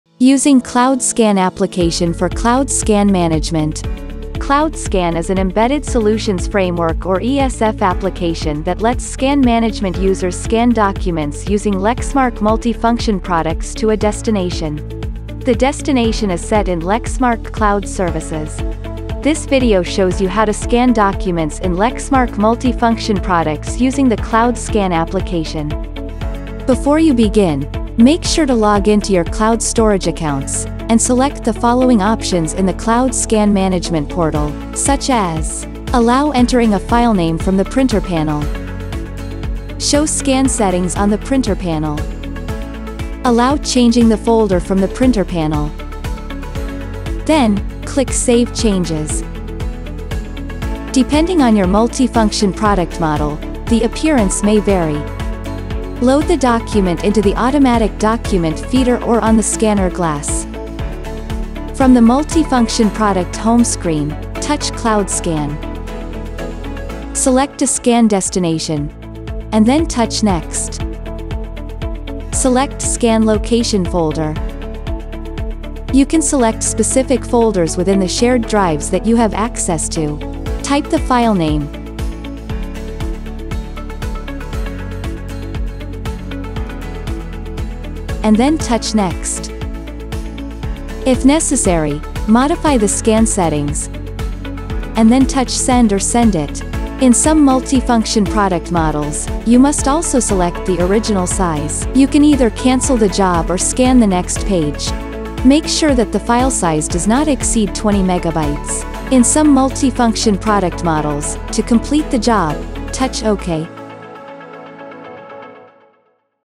Lexmark Cloud Scan Management—Using Cloud Scan application for Cloud Scan Management
Cloud Scan is an eSF application that lets Scan Management users scan documents using Lexmark™ multifunction products to a destination. For more information on the application, go